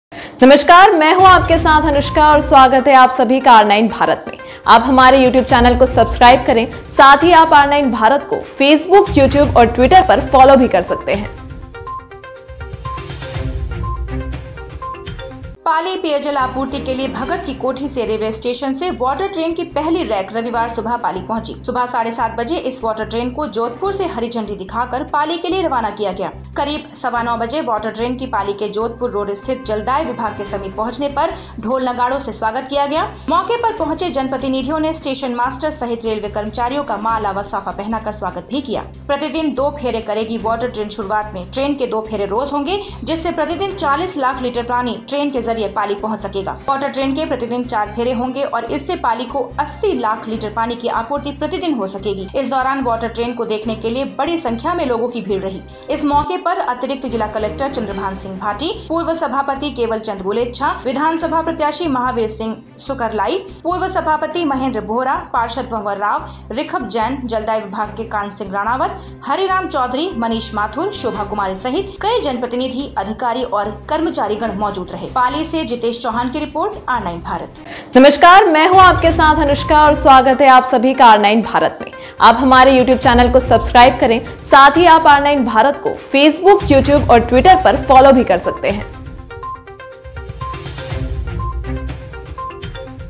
जोधपुर से पाली पहुंची 40 वैगन में 20 लाख लीटर वाटर ट्रेन, शहरवासियों में दिखी खुशी।
जितेश चौहान पाली